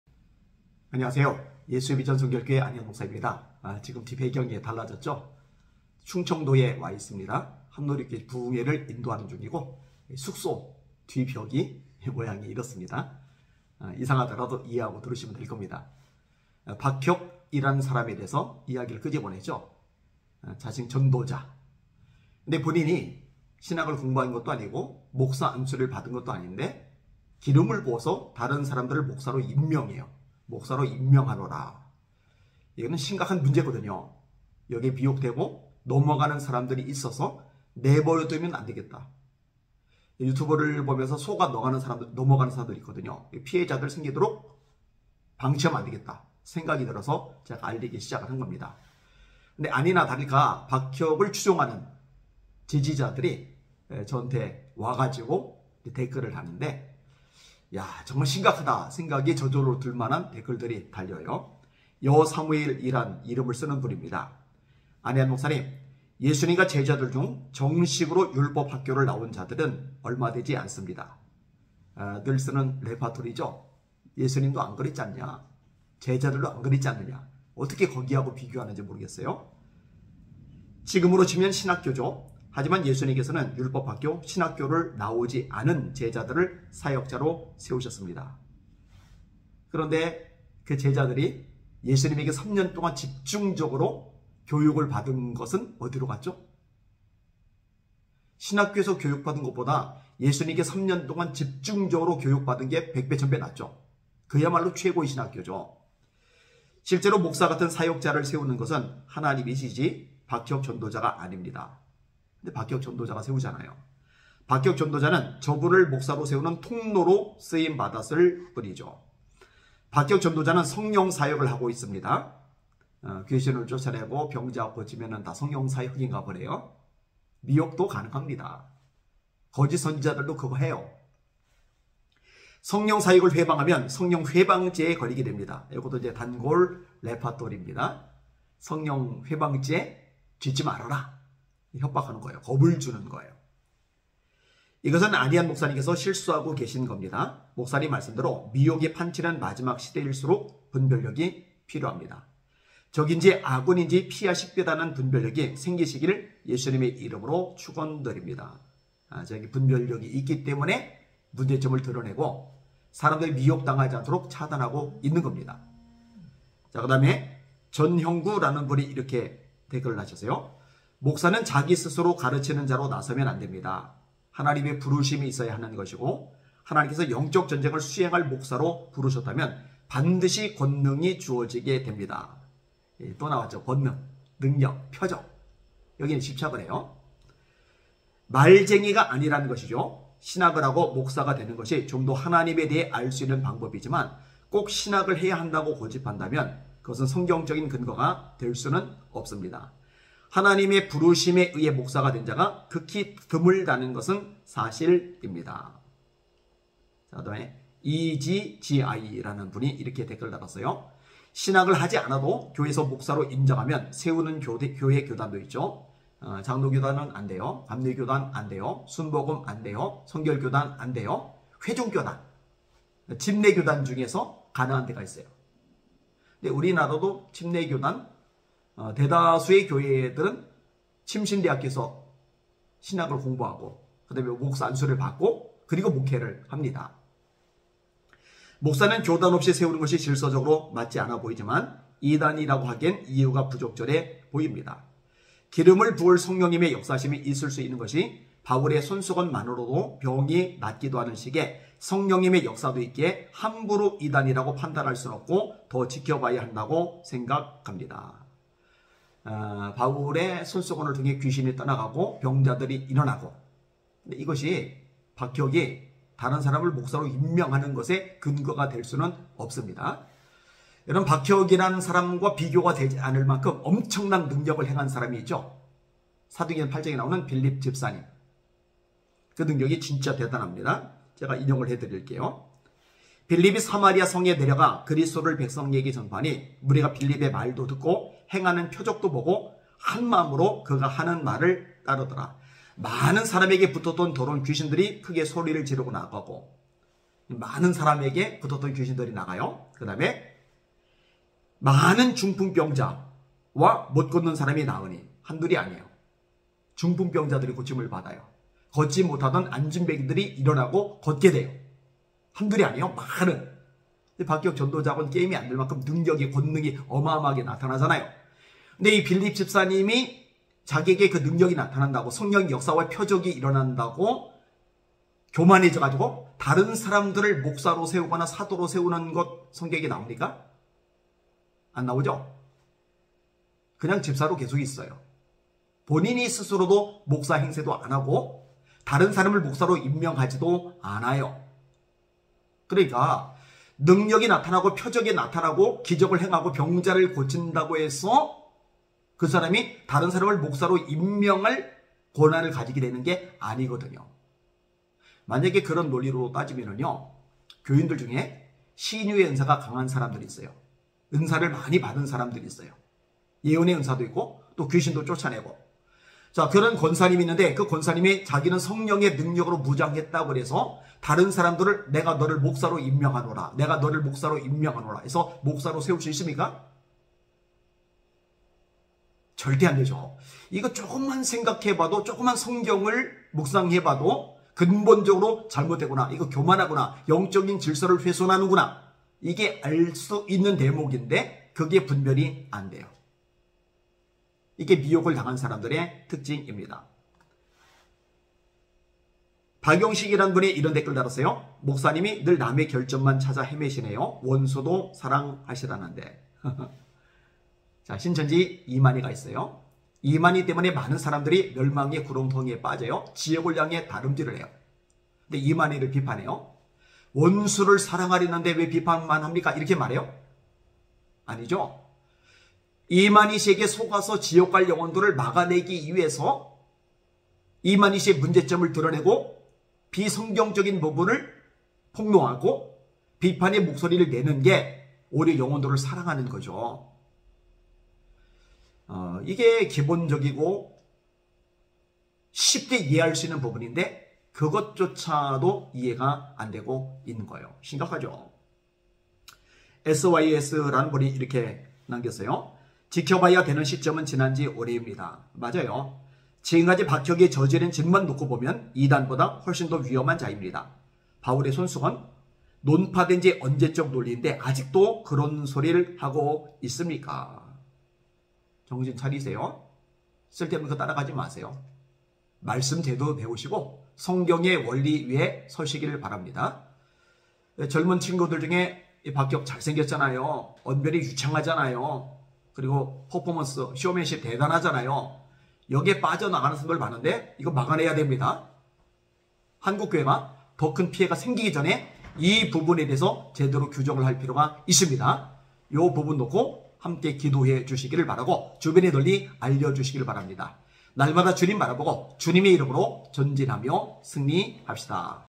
박혁의 미혹이 무서운 이유가 잘 드러나죠? 추종자들의 문제점들/ 안희환목사/ 예수비전성결교회
박혁의 미혹이 무서운 이유를 보고 계십니다. 추종자들의 문제점들/ 안희환목사/ 예수비전성결교회